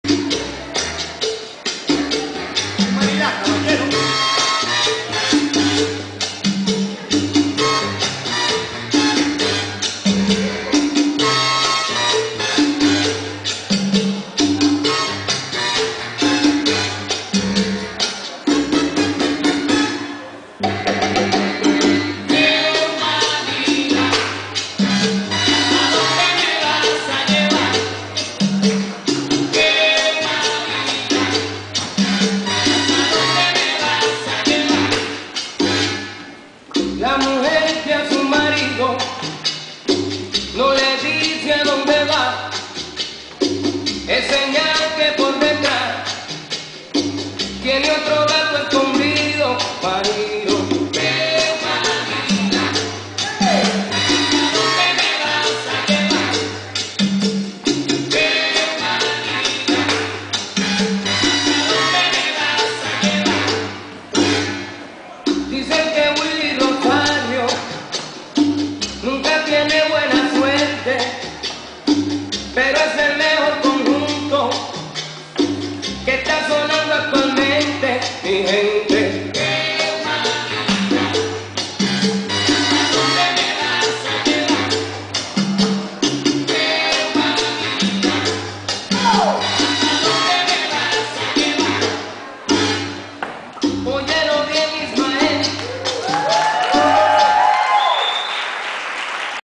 Francisco Vazquez - Cha Cha Shine (w/ music) - Fanta Festival 2012
This is a cha cha shine workshop with Francisco Vazquez from Fanta Dance Festival Sofia 2012.